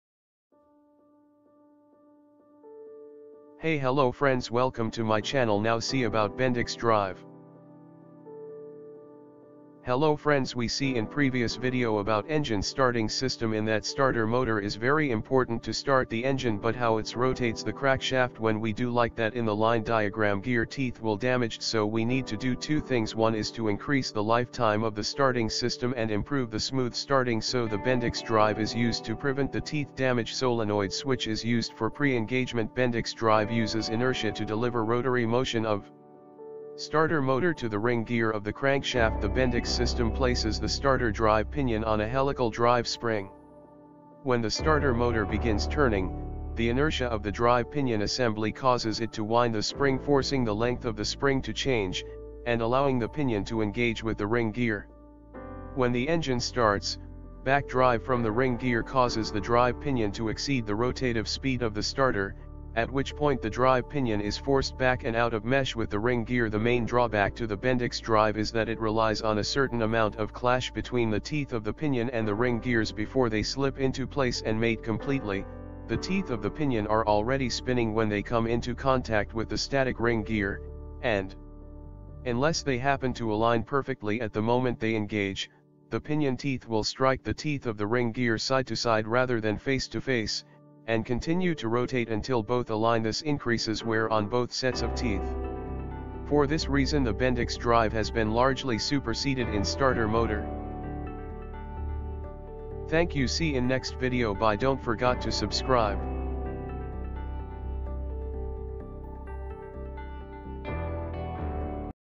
Bendix Drive |Explanation |Raghu Lesnar
The Bendix system places the starter drive pinion on a helical drive spring. When the starter motor begins turning, the inertia of the drive pinion assembly causes it to wind the spring forcing the length of the spring to change, and allowing the pinion to engage with the ring gear. When the engine starts, backdrive from the ring gear causes the drive pinion to exceed the rotative speed of the starter, at which point the drive pinion is forced back and out of mesh with the ring gear.

The main drawback to the Bendix drive is that it relies on a certain amount of "clash" between the teeth of the pinion and the ring gears before they slip into place and mate completely; the teeth of the pinion are already spinning when they come into contact with the static ring gear, and unless they happen to align perfectly at the moment they engage, the pinion teeth will strike the teeth of the ring gear side-to-side rather than face-to-face, and continue to rotate until both align. This increases wear on both sets of teeth. For this reason the Bendix drive has been largely superseded in starter motor design by the pre-engagement system using a solenoid.